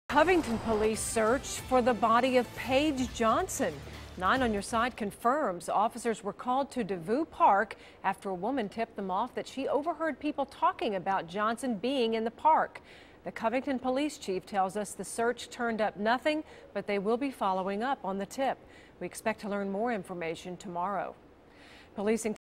Officers confirm search for Paige Johnson in Covington park
The search took place at Devou Park after a woman reported a tip to police.